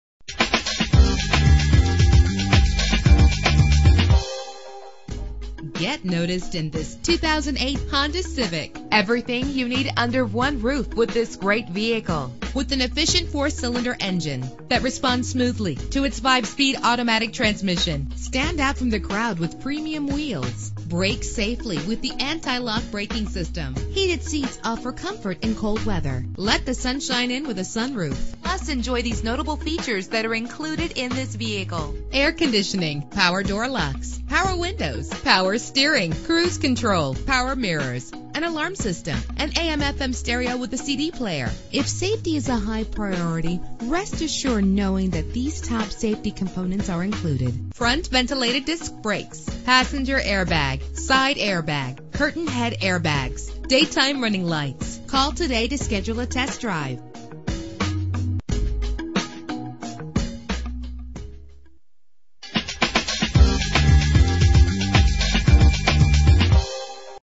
2008 Honda Civic - Denville NJ
Year: 2008
Make: Honda
Model: Civic
Trim: EX-L
Engine: 1.8 liter inline 4 cylinder 16 valve
Transmission: 5-Speed Automatic
Color: Black
Mileage: 35244
Address:
3166 Route 10